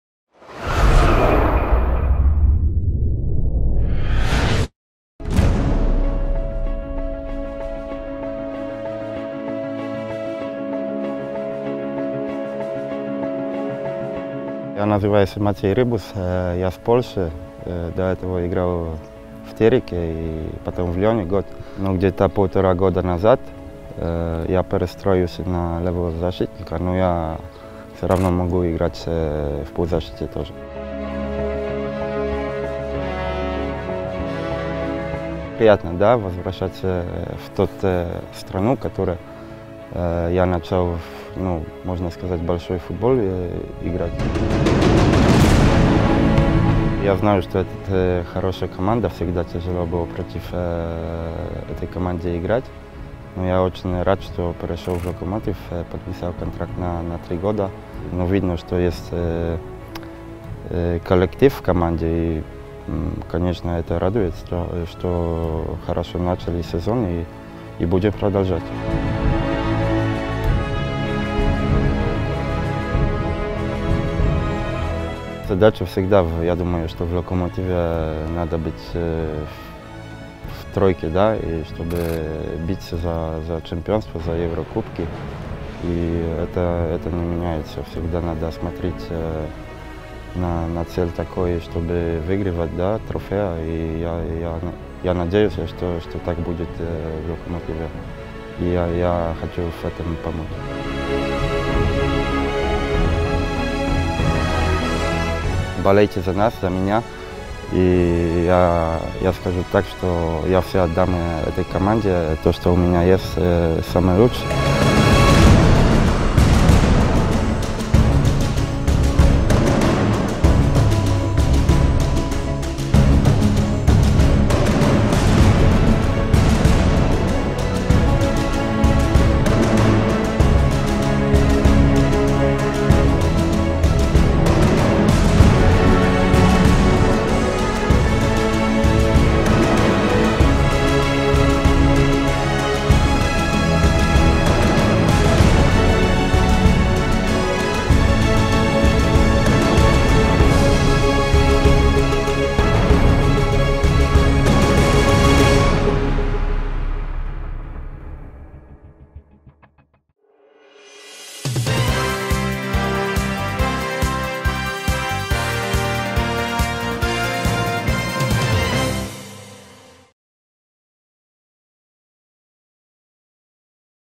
Мацей Рыбус. Представление новичка
Полузащитник Мацей Рыбус прибыл в расположение «Локомотива», провел первую тренировку и рассказал «Локо-ТВ», что он ждет от новой команды и нового сезона.